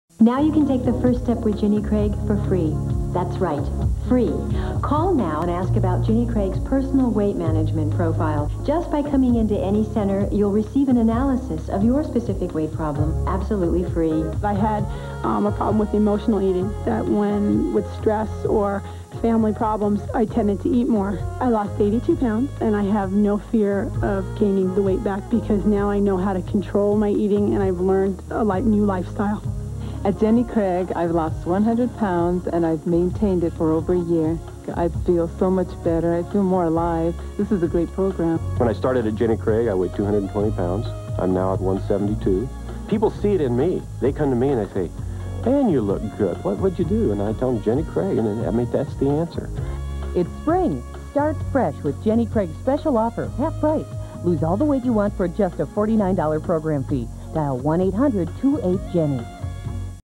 Jenny Craig - Personal Weight Management Profile Commercial (1994)
Aired On: FOX | KMSP-TV 9 | Friday Prime Movie - Sesame Street Presents: Follow That Bird | 4/8/94